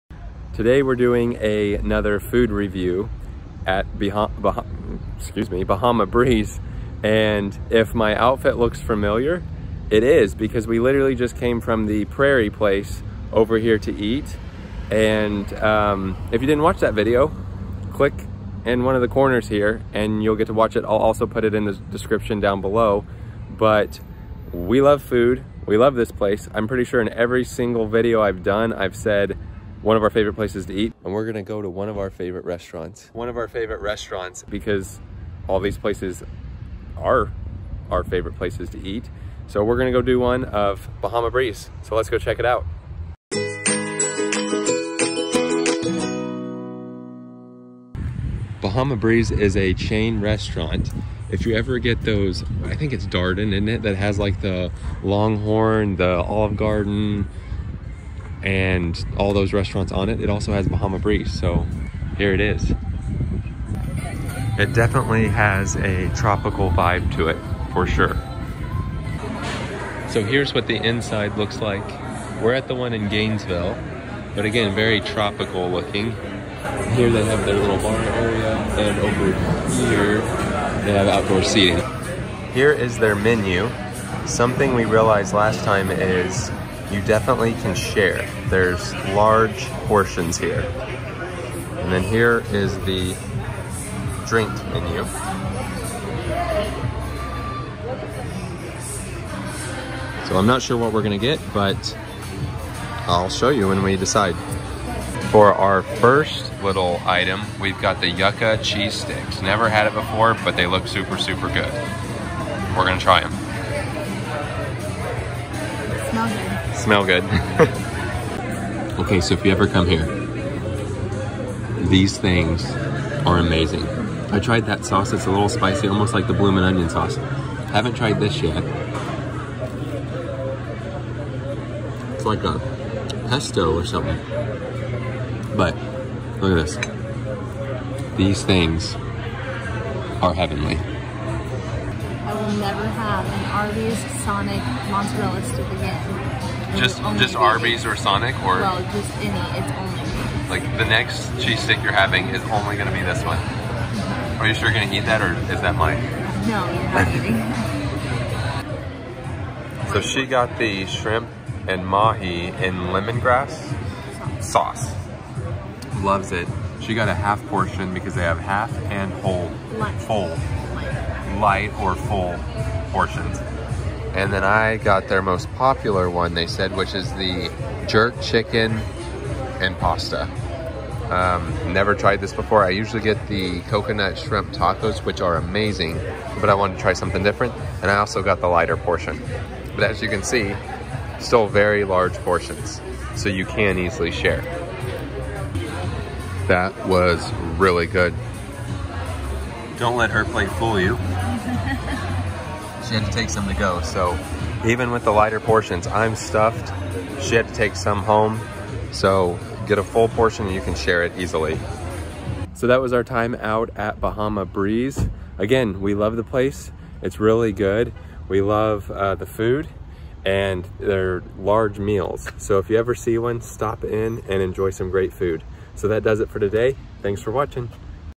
Bahama Breeze Island Grille | Places to eat at in Florida | FL Restaurant Review
In today's video, we go to a very popular chain restaurant in Florida, Bahama Breeze. We went to the one in Gainesville, FL and it was fantastic! The staff is always nice and the food is always great. Bahama Breeze is part of the Darden Company restaurants and never disappoints! We decided to get the Yuca Cheese Sticks and we both said that they were some of the best that we have ever had. There portions are huge so they give you the option for a full amount or lighter portion. If you are getting a full amount, you can easily share! Lighter portions still will fill you up. Tember got the Shrimp & Mahi with Lemongrass Sauce and this is her go to. Alex got the Jerk Chicken Pasta and thought it was great! All in all, we love visiting Bahama Breeze and it is so good. If you ever see one, stop in and check it out. God Bless!

Other Food Reviews!